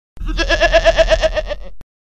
Goat funny-Royalty free sound effect(HD)
About: A billy goat bleat sound effect. this is a long bleat and great for farm, goat, or mountain sounds.